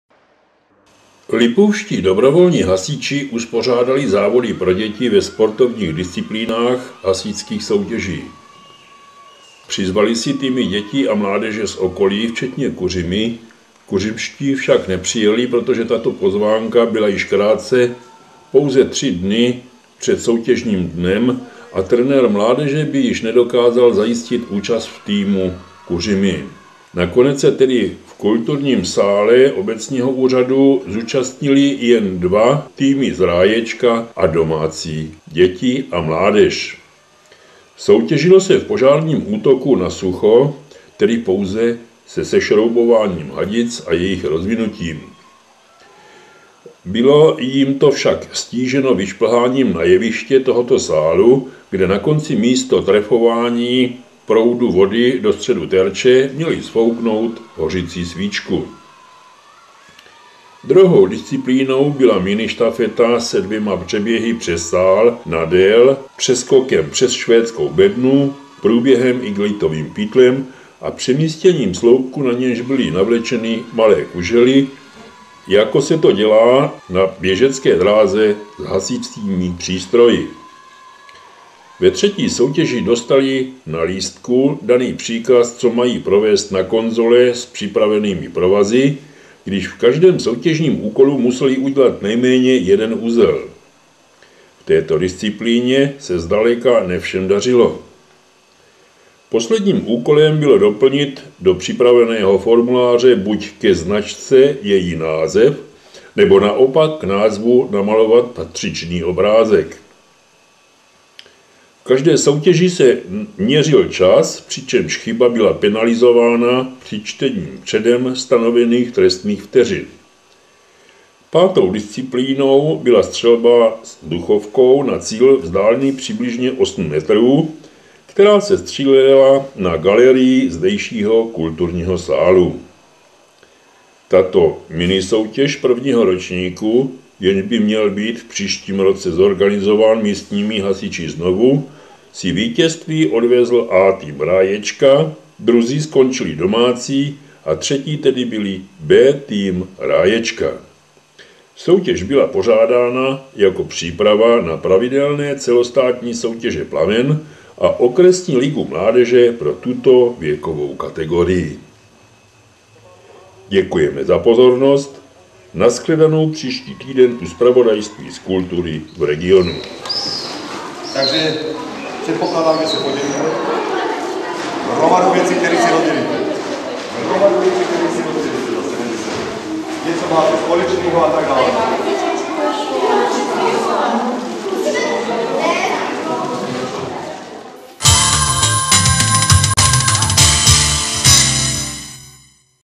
Hasiči SDH Lipůvka pořádali závody mládeže.mpg
Hasiči SDH Lipůvka pořádali závody mládeže za účasti SDH Ráječko